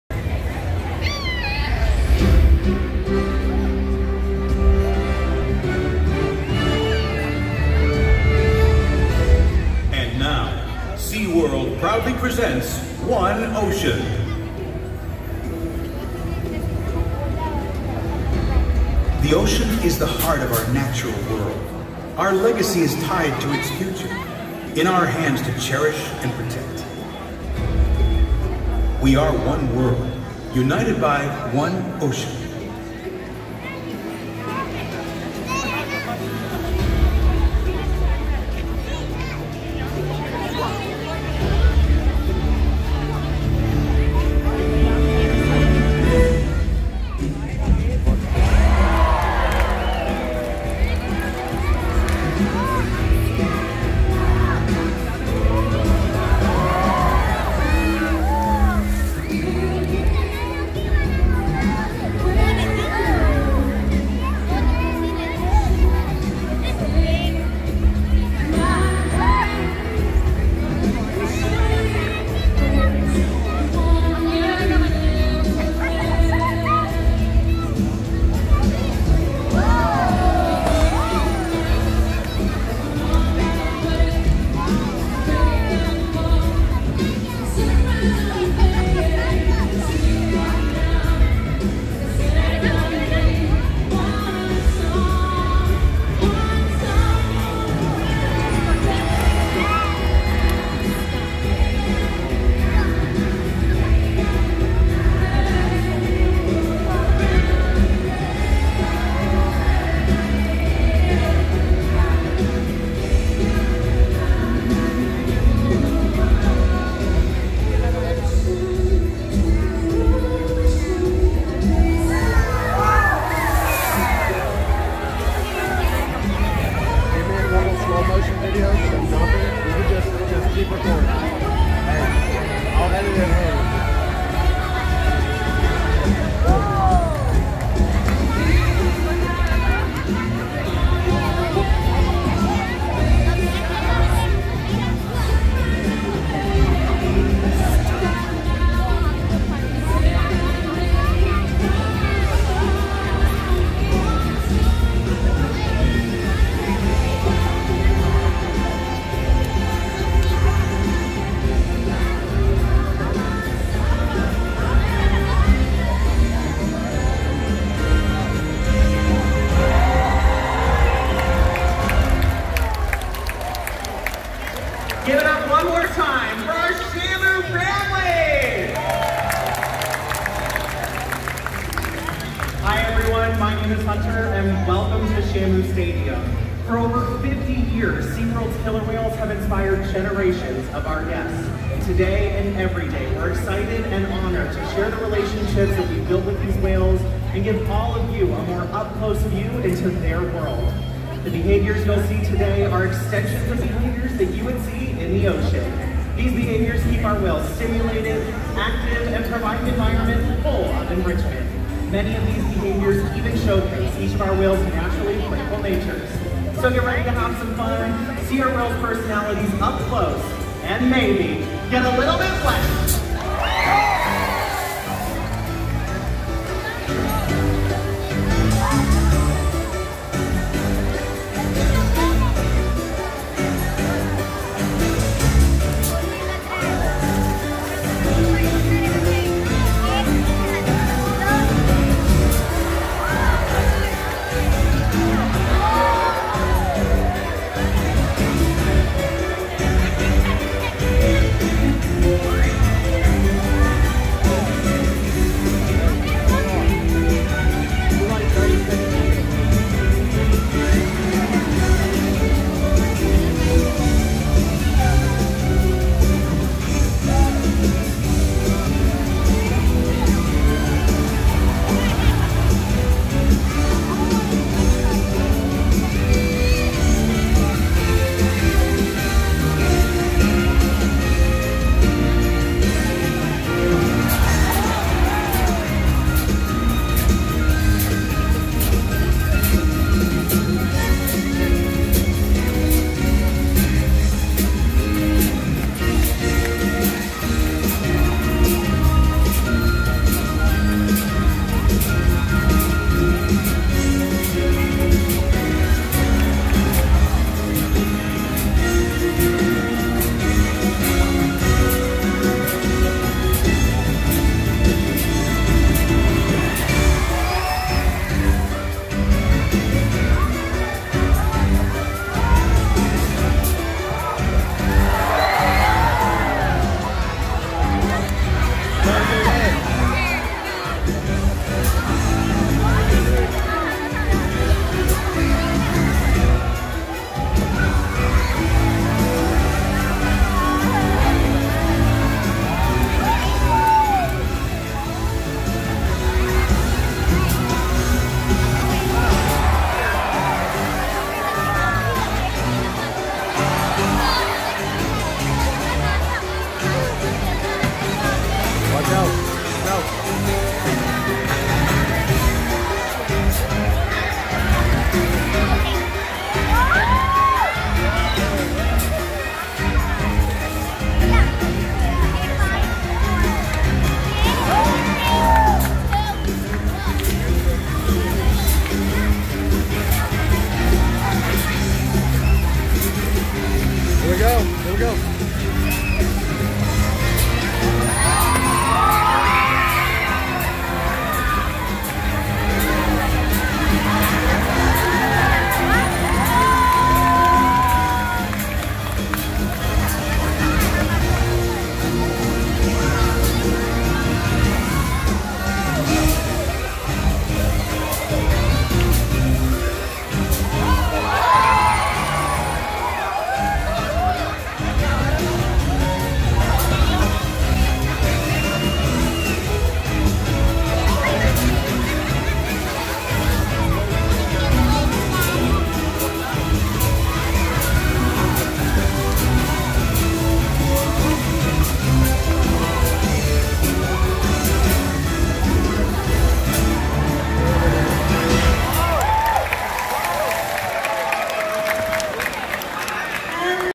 SeaWorld Orlando One Ocean Performance
My daughter's birthday trip went great!